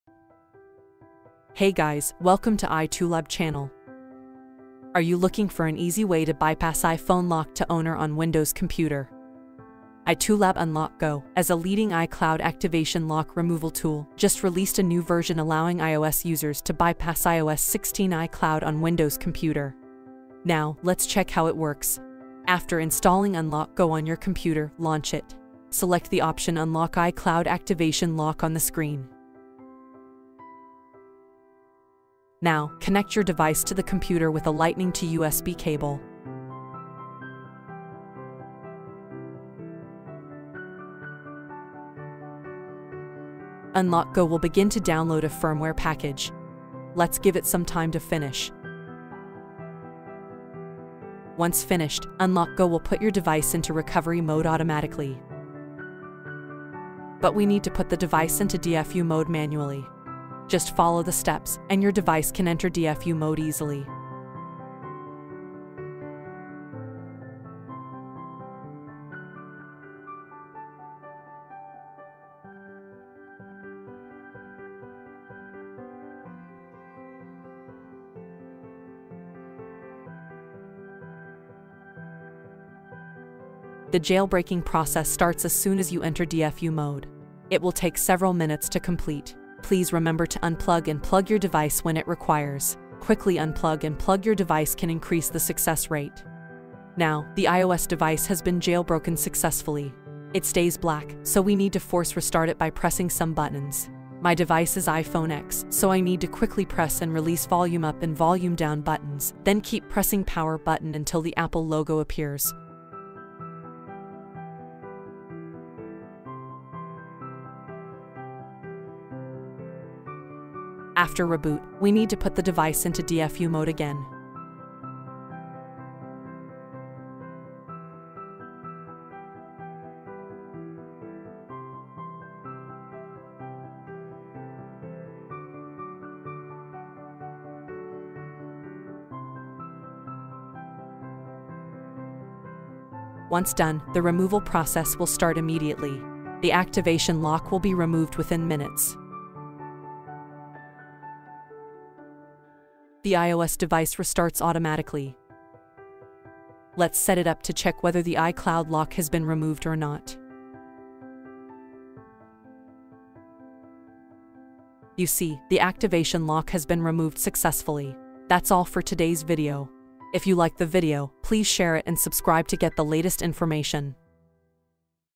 [NEW UPDATE] iOS 16 iCloud Bypass on Windows | Easy Method with iToolab UnlockGo
📢Good news for Windows users!
iToolab UnlockGo just released a new version, which allows Windows users to remove iCloud Activation Lock on iOS 16 devices.

Timestamp
00:00 Intro
00:20 Launch iToolab UnlockGo
00:35 Connect the iDevice to the computer
00:44 Download the firmware package
01:24 Jailbreak iOS 16 device
02:22 Unlock iCloud on iOS 16

⚠Important notice:
2. After you've bypassed iPhone locked to owner, don't set screen passcode on your iPhone. Or it will be locked again.